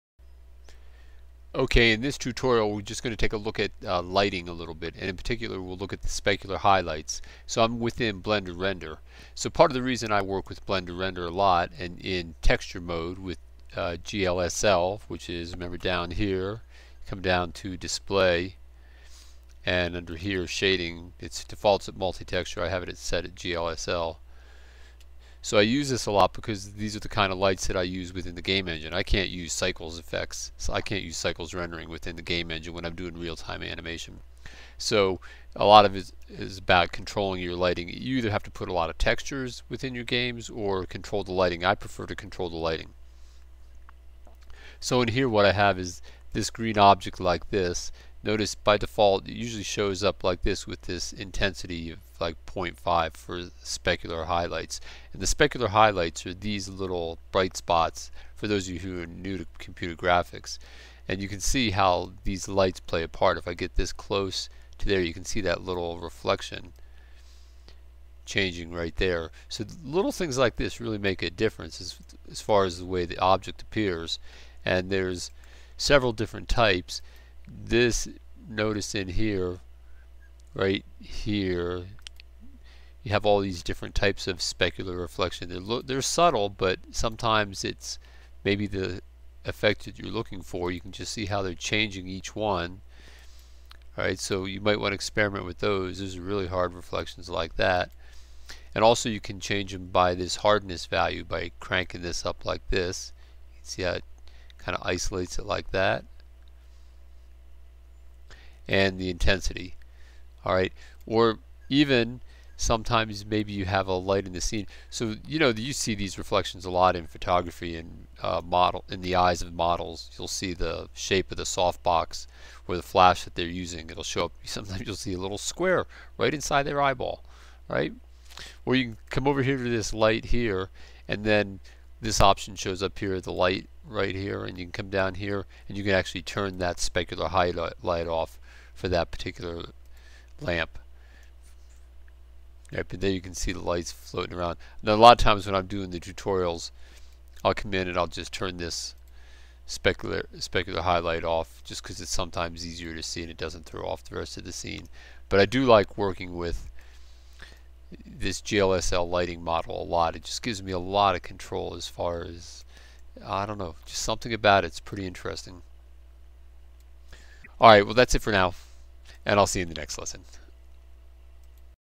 Blender Tutorial - Controlling Shiny Specular Highlights on your Objects
In this Blender tutorial, we look at some specific details that will allow you to easily control the shiny specular highlights on the objects within your scene.